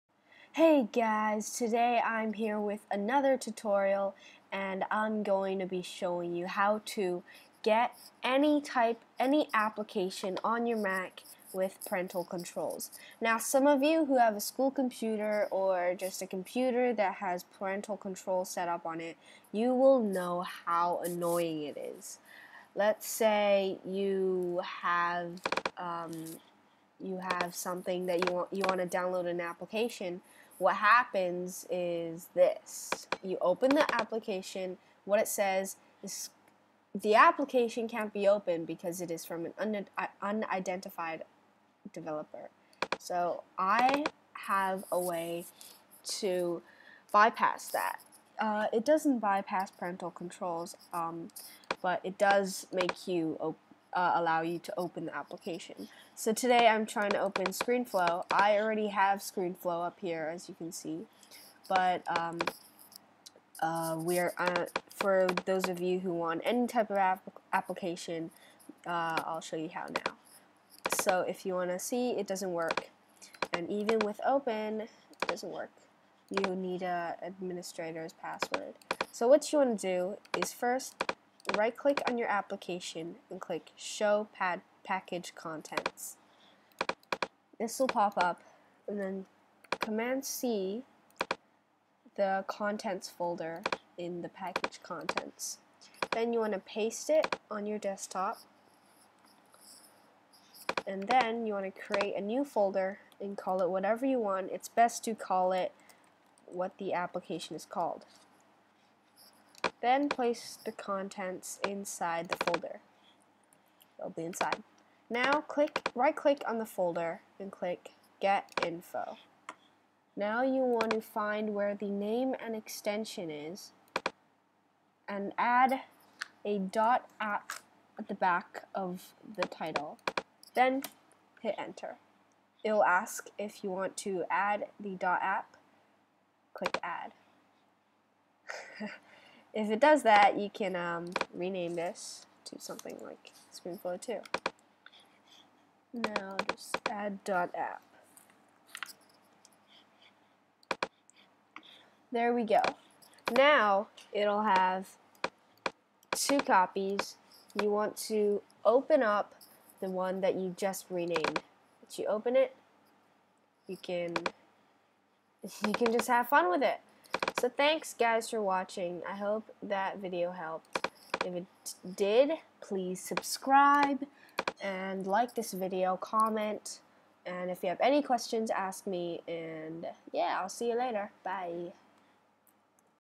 How to get any app on your computer (mac) with parental controls
Ever had trouble with Parental controls and want to download an application but you can never open it? Today I'm here with a tutorial teaching you how to bypass the application restrictions and use whatever app/open whatever app you want.